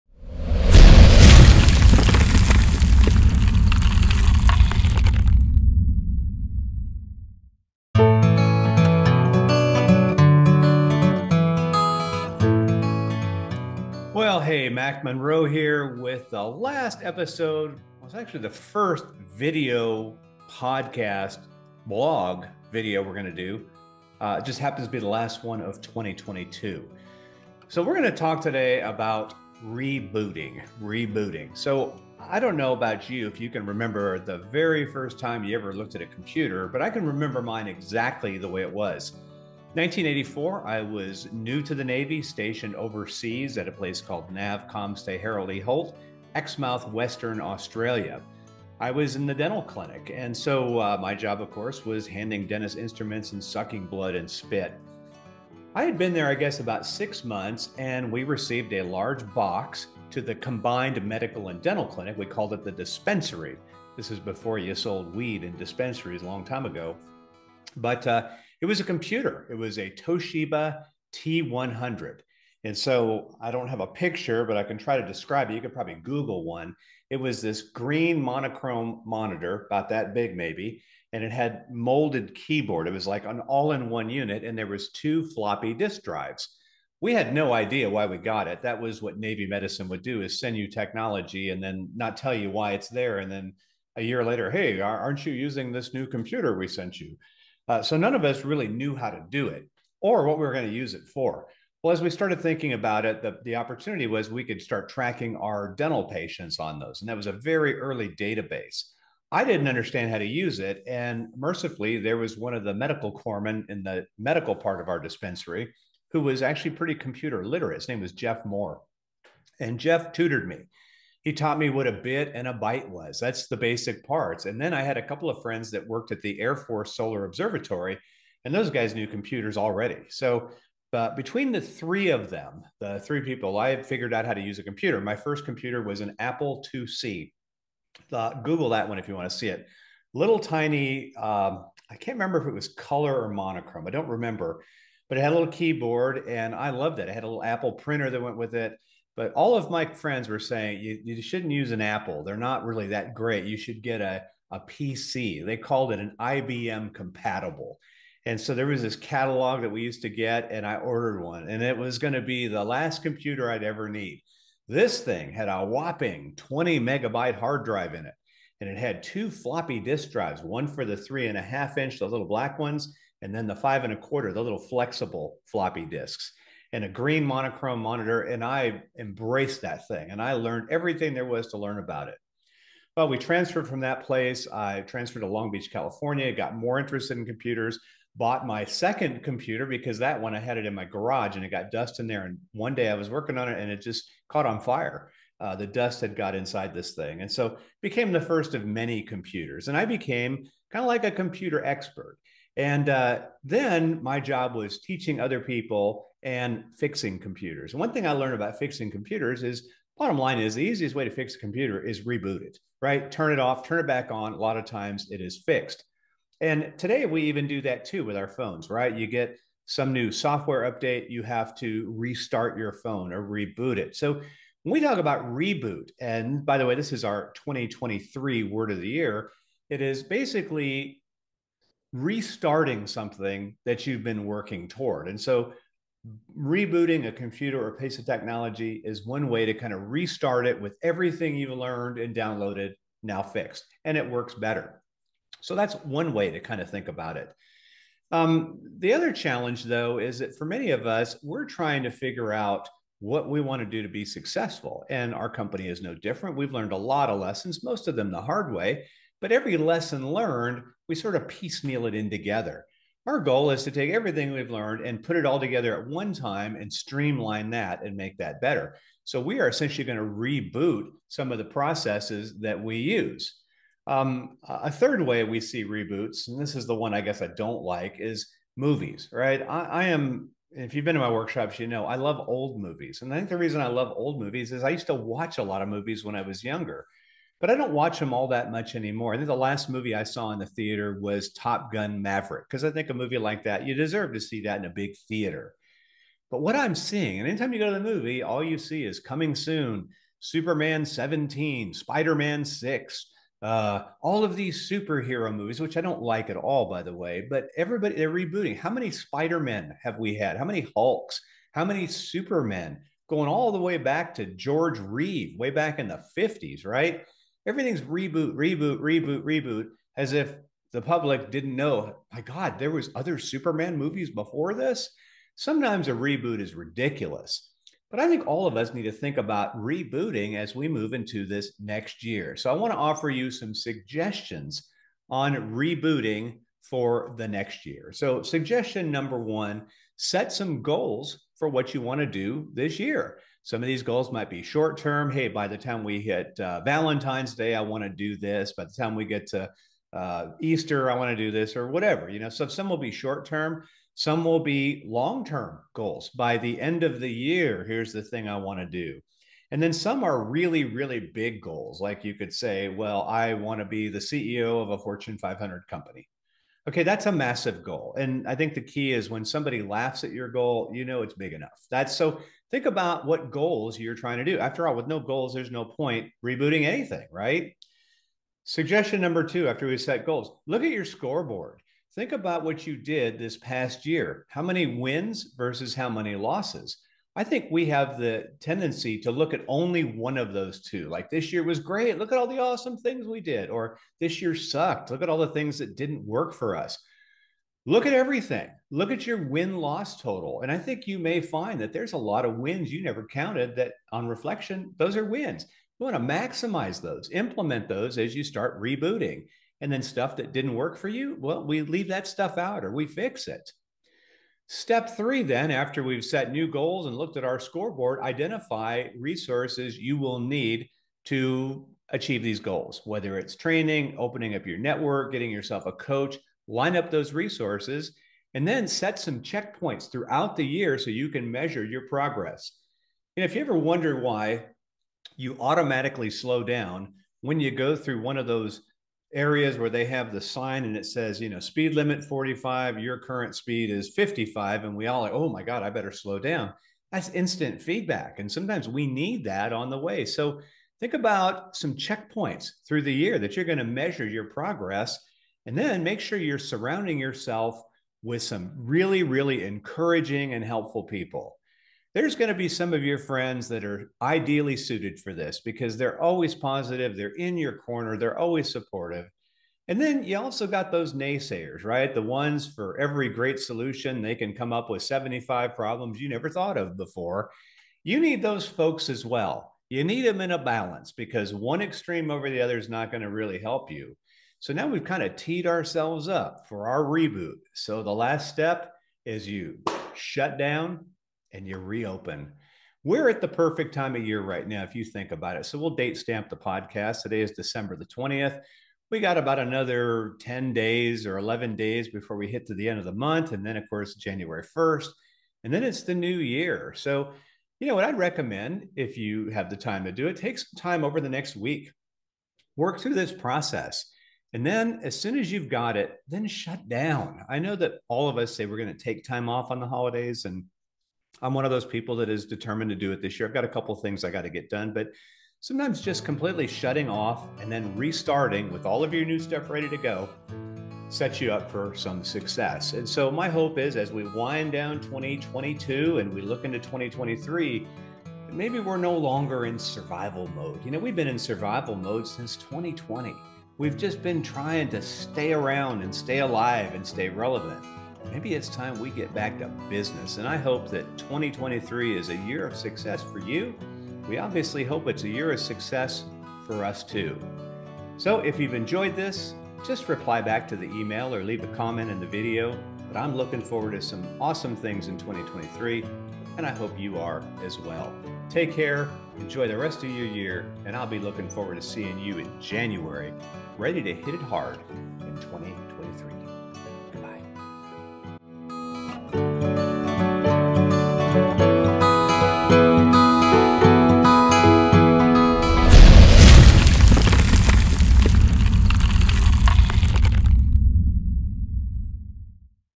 The Reboot
We're at the end of the year. This is when a lot of folks set resolutions, or, in our case, a Word of the Year.  It's REBOOT for us.  I'll walk you through it in this video.